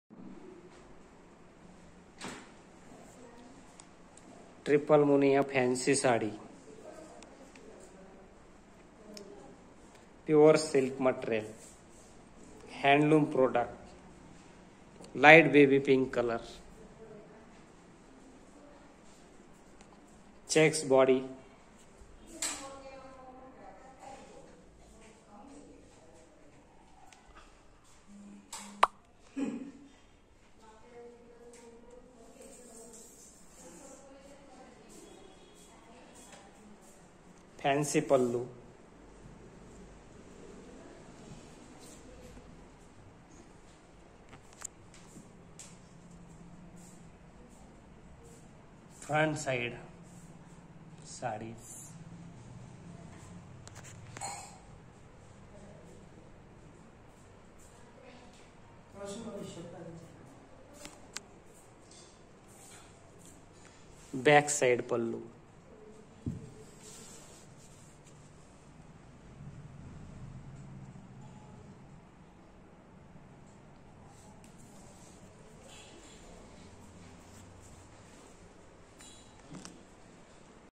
pure silk paithani 📲 8177941392
Fabrikh type pure silk paithani

Trippal Muniya brocket paithani

Golden jari pura handloom products

checks body

clr/- light Baby pink combination

parrot fancy pallu

Available limited time

Book fast

Note/-

Colour May by slightly differ to due phone camera And light effects

100%Handweaving